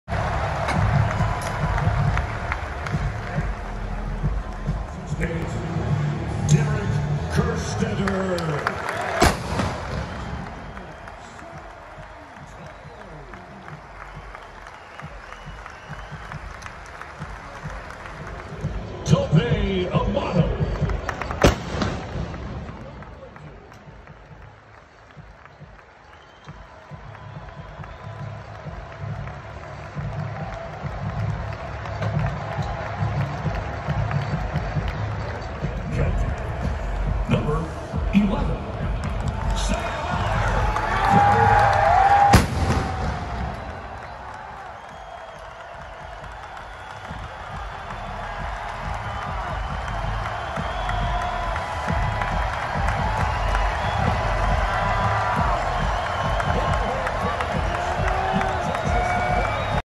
Silver Spurs fire Smokey the Cannon for 2020 Texas Football Silver Spur Seniors
Silver Spurs fire Smokey the Cannon on 2020 Senior Day for Silver Spurs Cade Brewer, Tope Imade, Derek Kerstetter & Sam Ehlinger. Silver Spurs on cannon: Thomas Fatjo, Will McLeroy, Cody Jones, Colby Davis